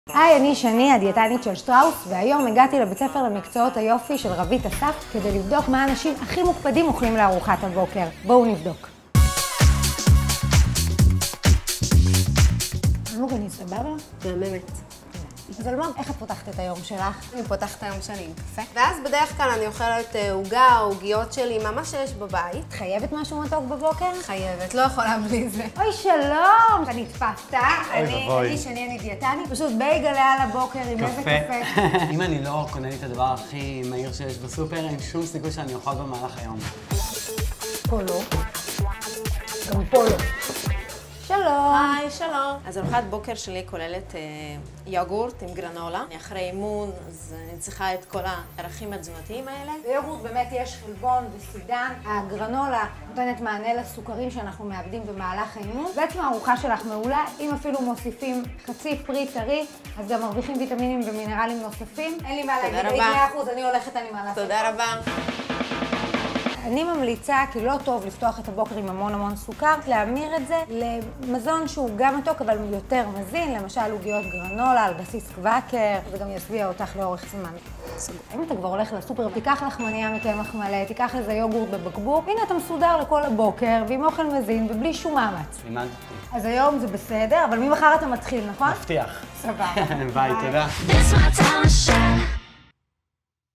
הפסקת אוכל עם דנונה- מה אוכלים האנשים הכי מתוקתקים שיש?
יצאנו לבדוק מה אוכלים האנשים הכי מוקפדים שיש, האם גם ארוחת הבוקר שלהם מוקפדת?